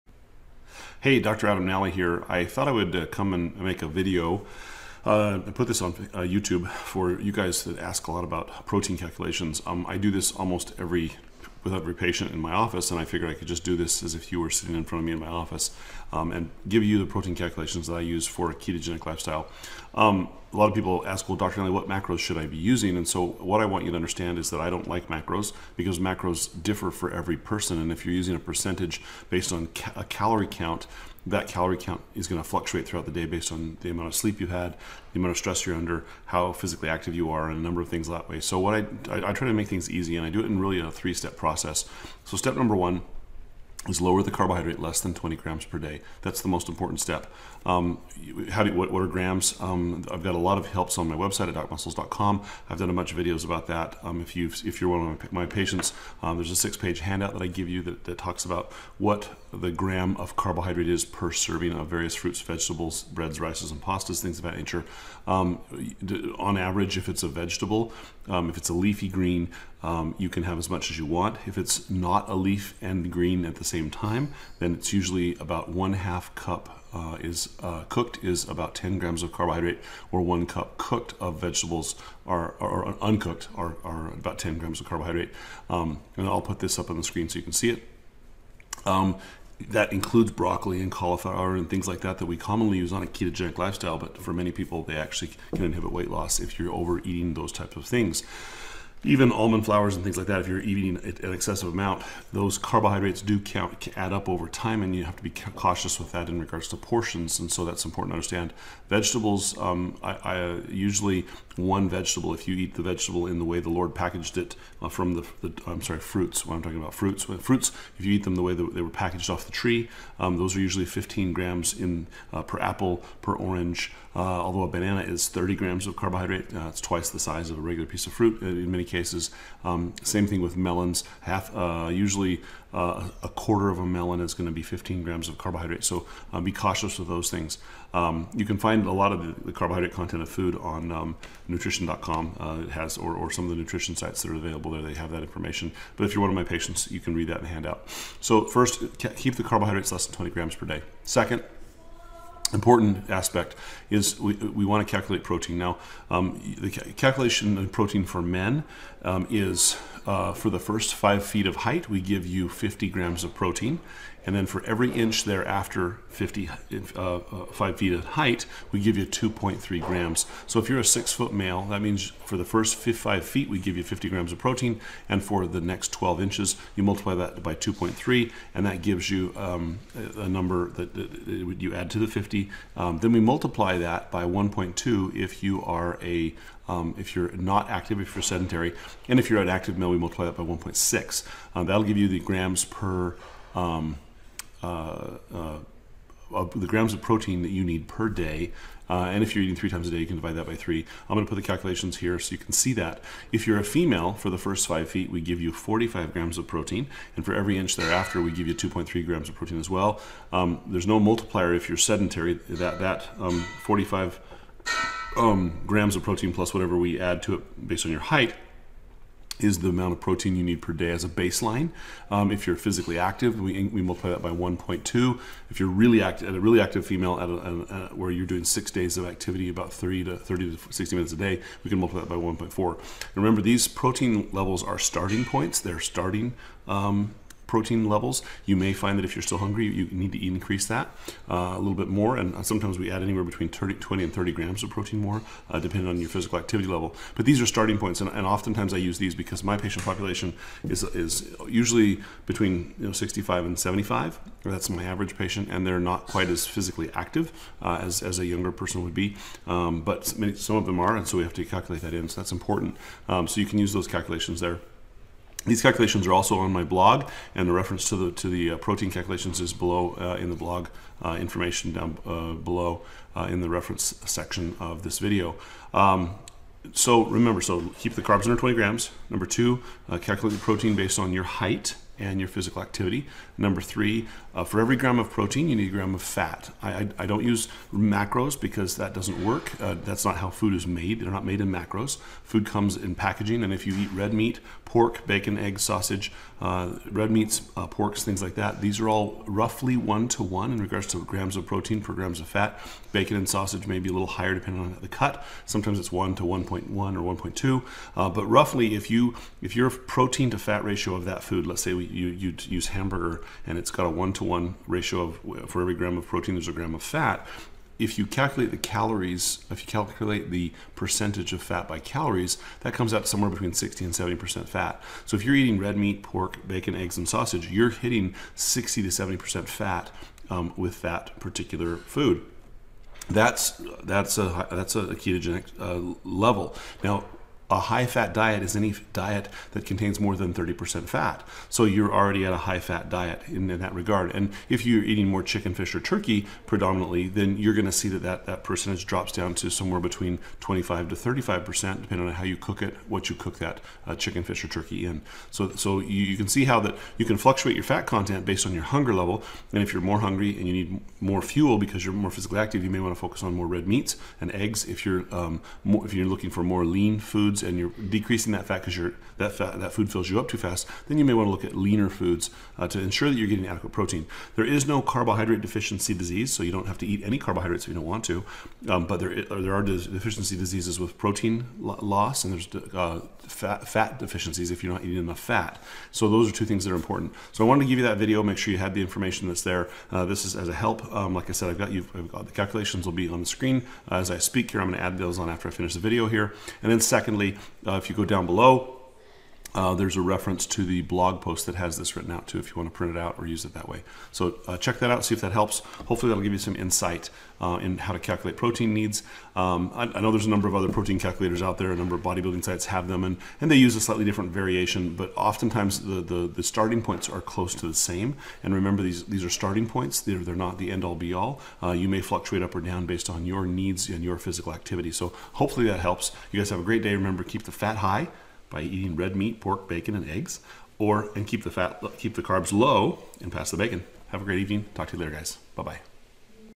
Calculating Protein Needs On A Ketogenic Lifestyle
Dr Nally discussed the three basic steps in calculating your nutrient and protein needs when starting or following a ketogenic lifestyle.